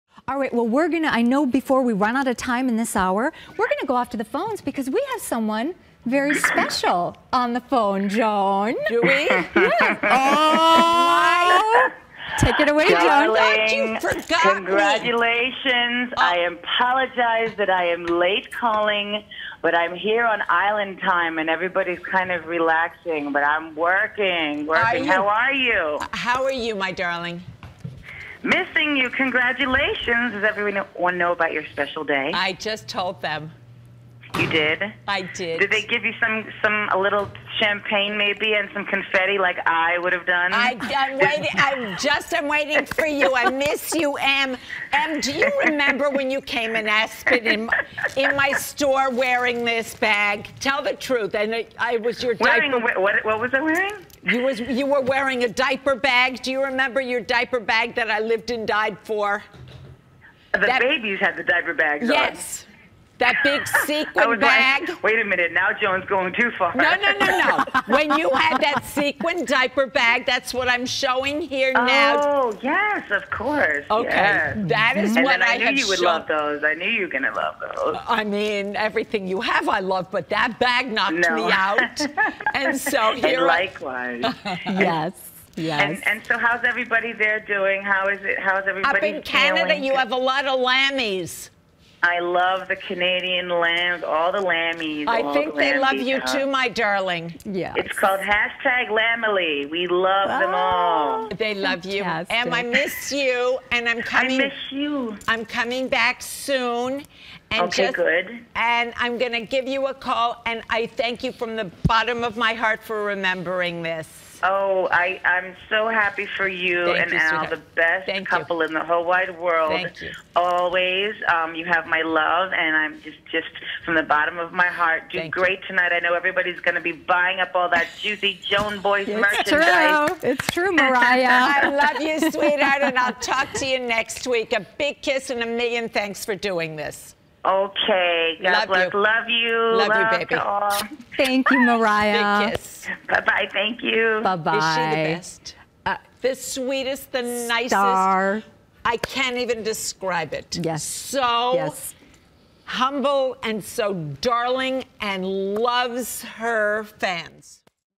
Mariah Carey Calls The Shopping Channel
Mariah Carey joined us for some Joan Boyce shopping! Mariah phoned TSC during our live show to chat about our Joan Boyce Sequin Tote & Makeup Bag TodaysShowstopper, which is also known as Mariah's "Mom Tote".